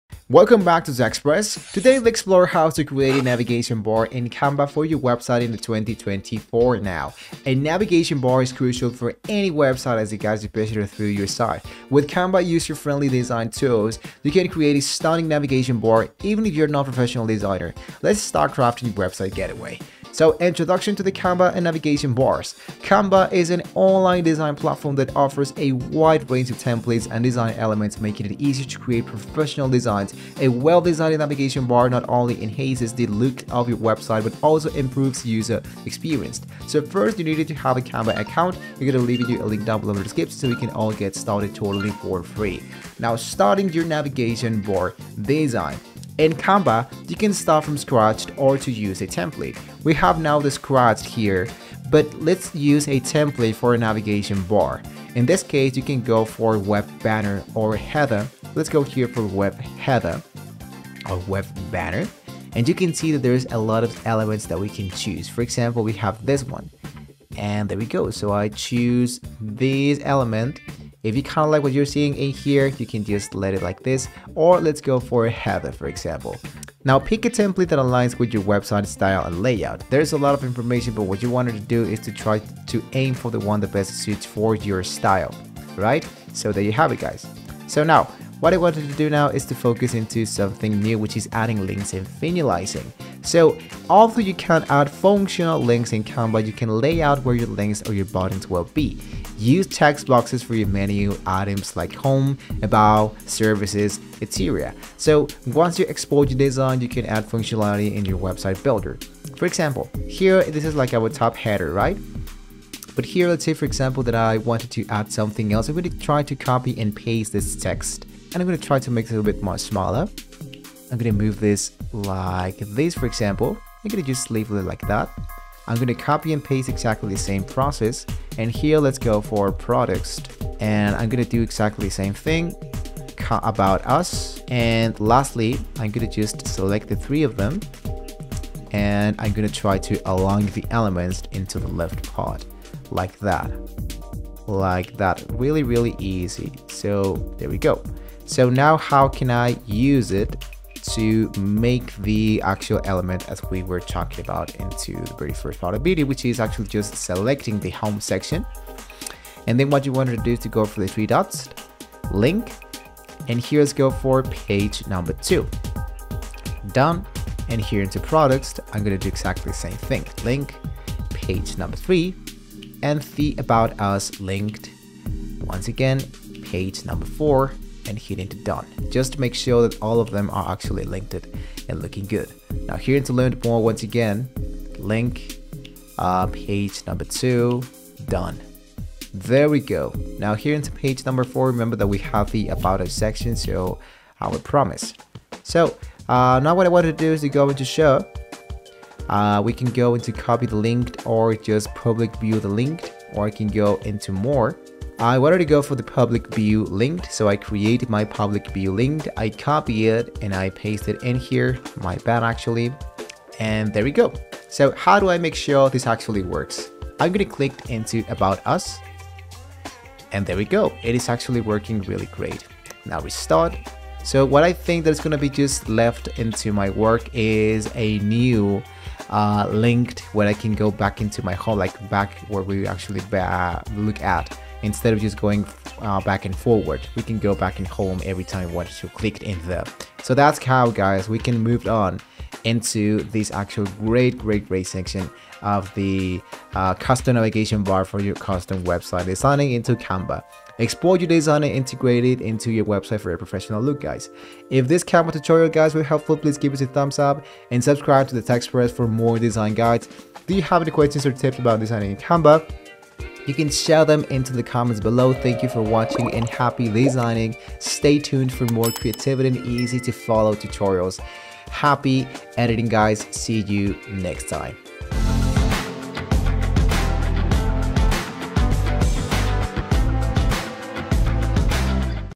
How To Create Navigation Bar In Canva 2026 (Canva Website Tutorial)
Creating a navigation bar in Canva is a straightforward process that can greatly enhance the user experience of your designs. To begin, click on the "Elements" tab and select "Shapes" from the dropdown menu. Then, choose a rectangle shape and drag it onto your design canvas. Next, customize the size and color of the rectangle to your liking.

After that, click on the "Text" tab and add your desired menu options. To make the navigation bar interactive, you can link each menu option to a specific page or section. Finally, arrange the elements of the navigation bar to your preferred layout. With these simple steps, you can create a professional and functional navigation bar in Canva.